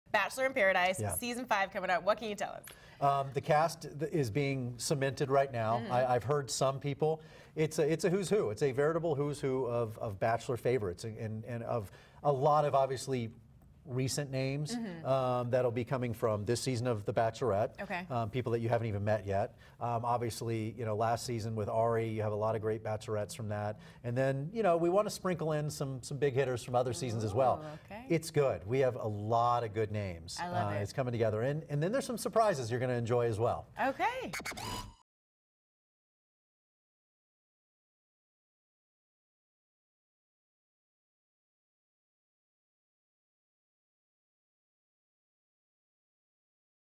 Chris Harrison Teases "Bachelor in Paradise" Season 5 Cast | E! Red Carpet & Award Shows
The "Bachelorette" host promises fan favorites, fresh faces and even some wildcards in the fifth season of "Bachelor in Paradise". Watch!